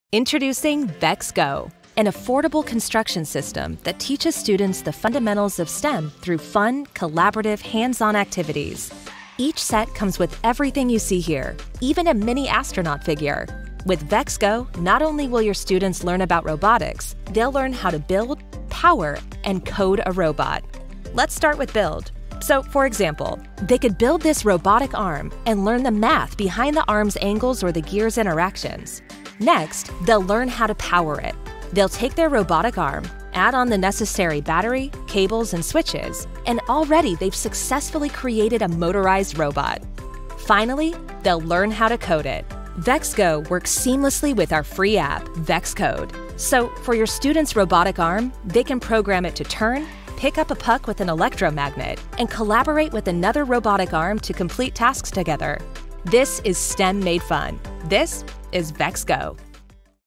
Představení VEX Go
Seznamte se s novinkou: robotickou stavebnicí VEX určenou pro 1. stupeň